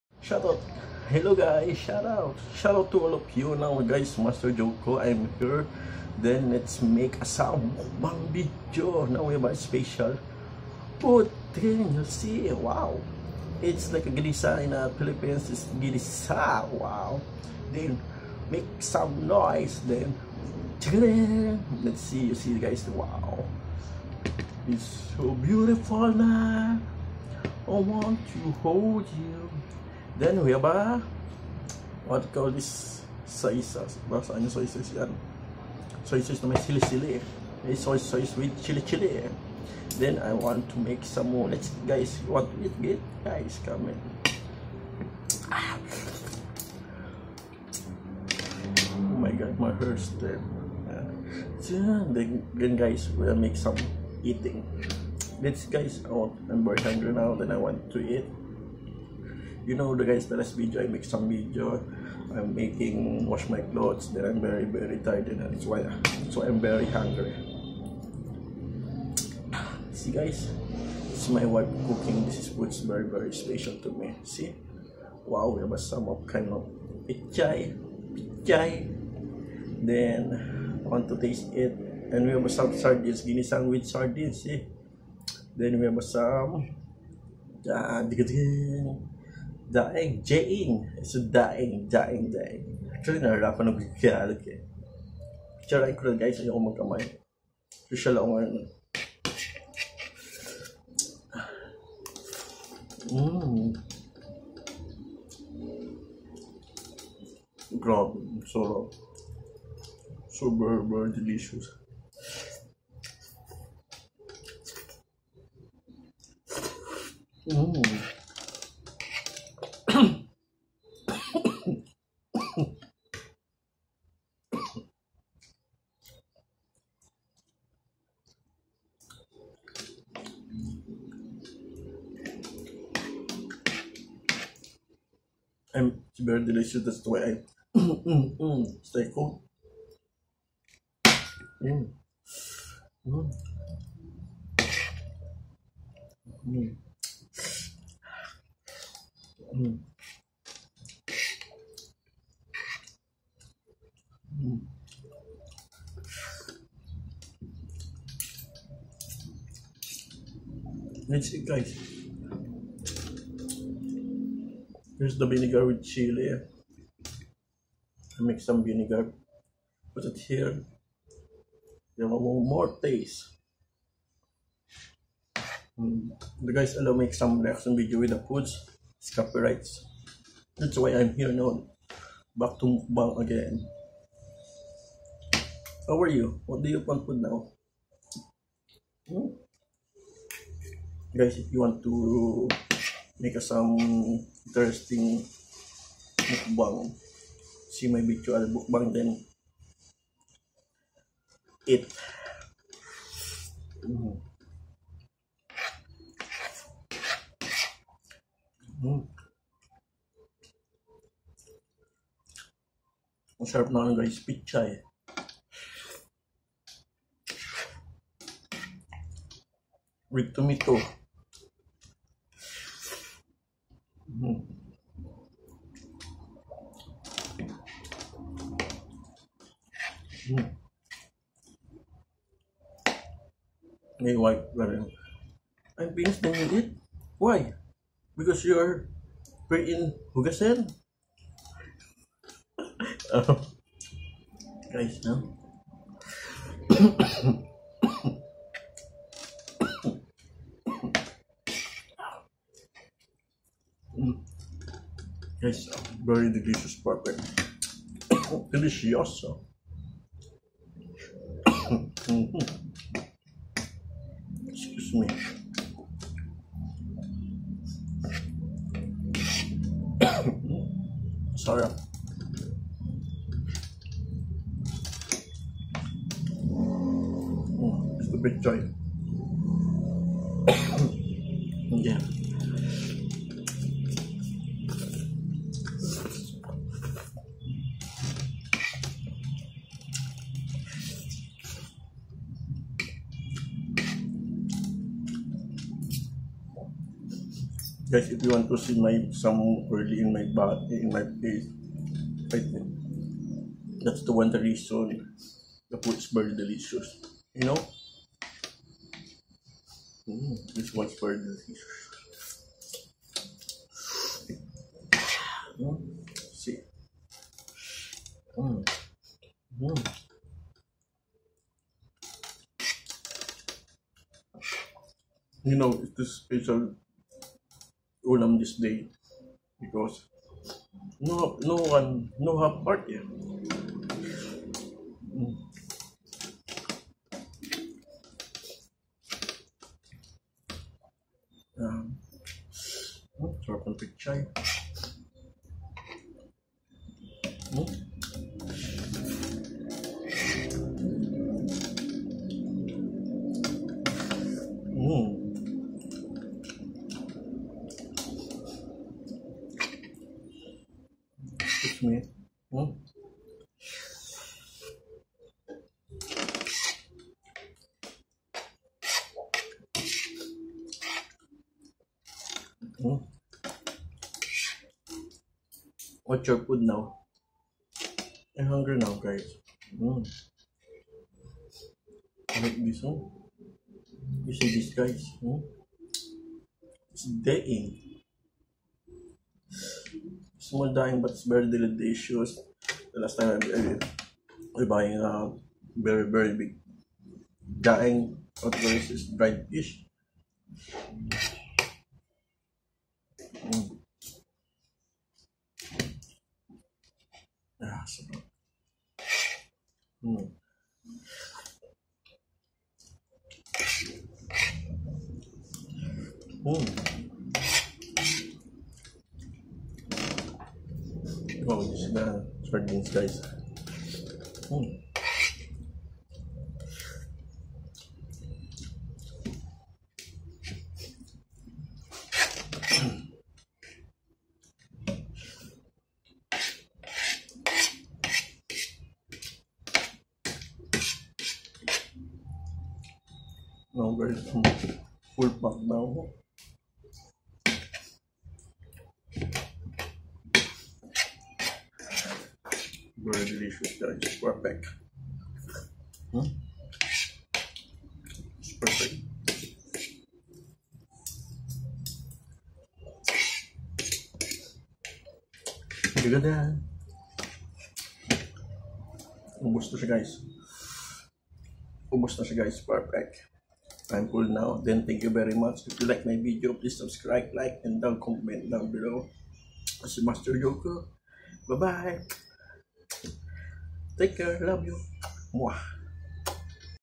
SARDINES WITH CABBAGE and TOMATO ASMR FILIPINO MUKBANG / PINOY FOODS
It's a sardines with vegetables like Tomato and Cabbage it's Pinoy foods yummy and delicious.